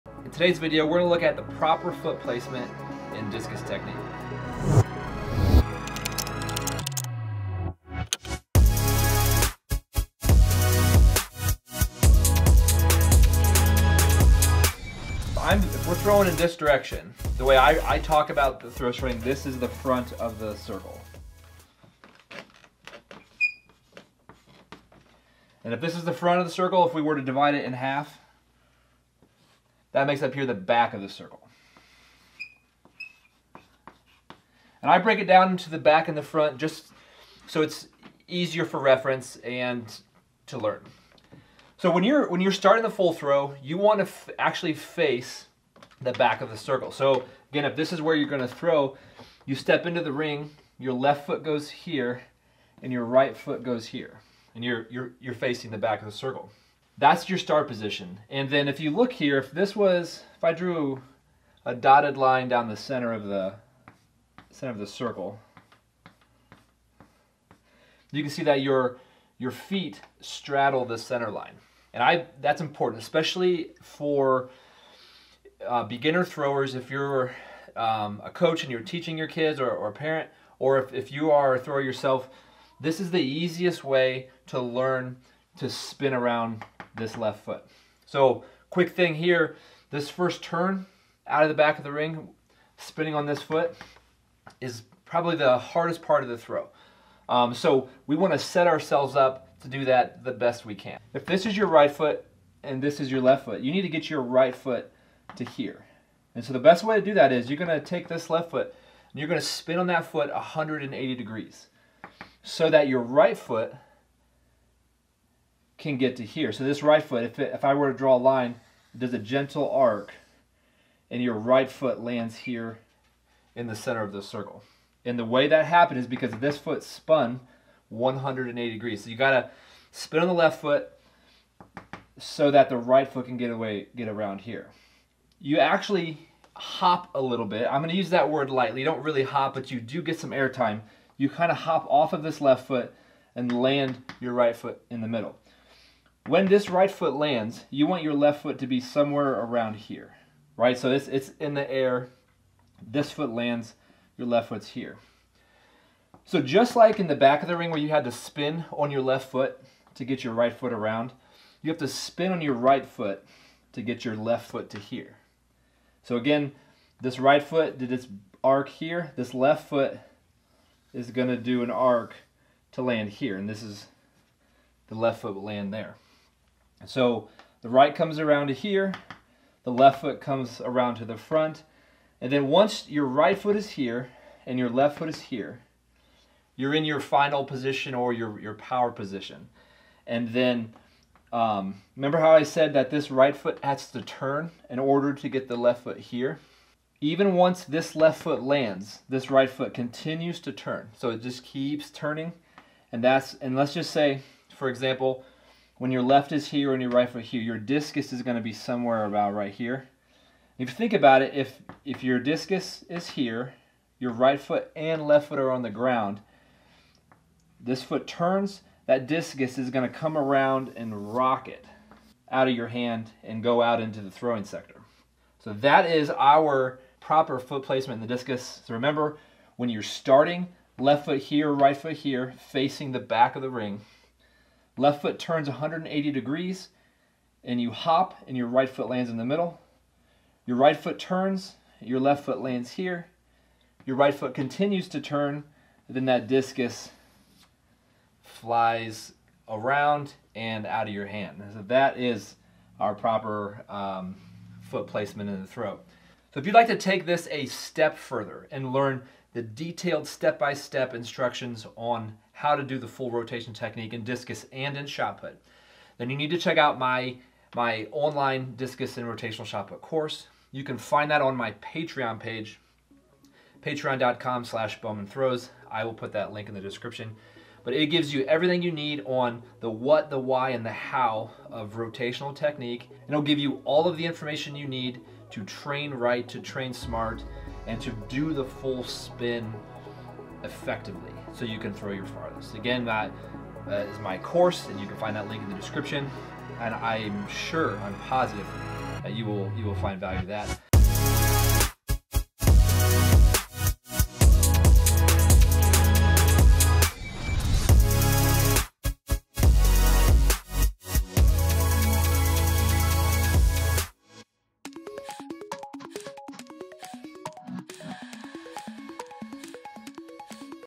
Basic Discus Throw Technique (the footwork)
Here's a look at the foot positions in the full spin technique... how to start the throw, where your feet need to go, and what they need to be doing. Enjoy!